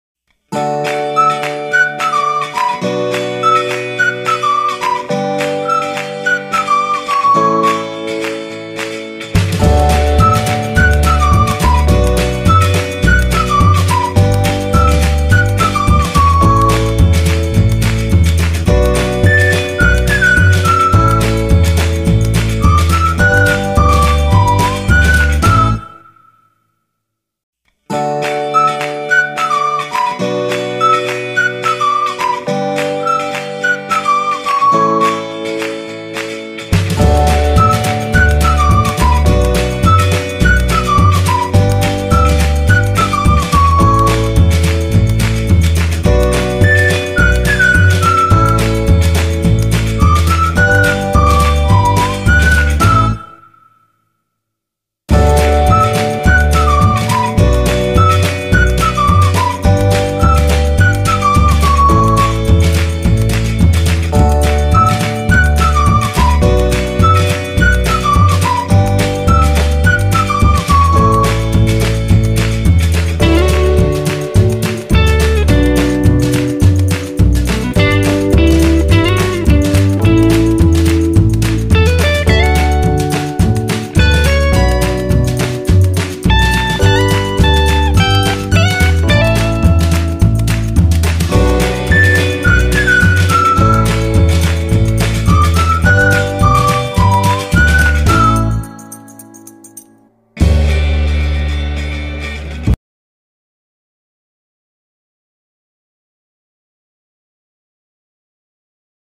TINGKAH KUCING BETINA BIRAHI MENGGODA KUCING OREN
tingkah lucu kucing betina birahi lagi menggoda jantan  ,,di pinggir sawah dengan asyiknya guling guling di depan kucing Oren kampung